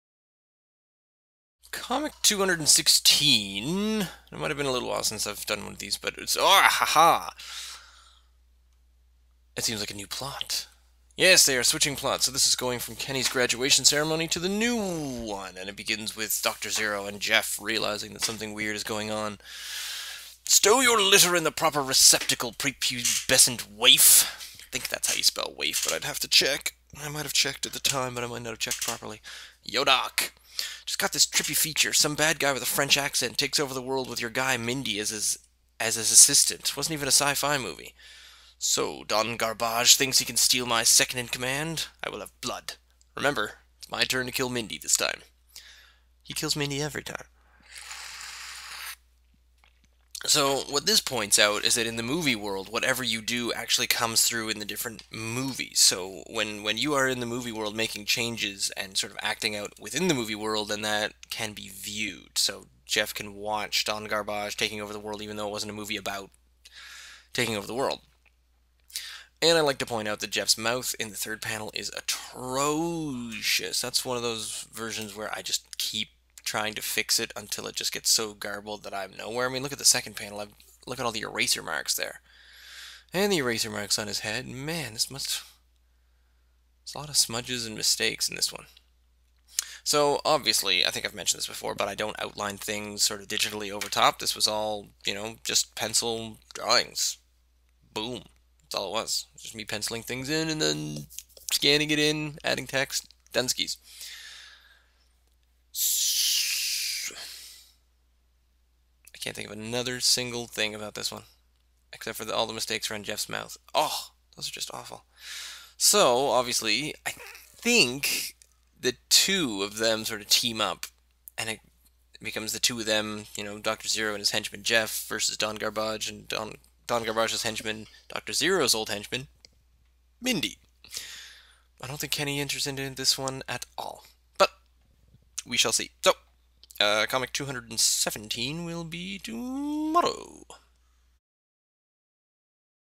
Intermission Commentary #216
Creator's commentary #216. In which I discuss my theories behind the movie world I invented for my own plot contrivances.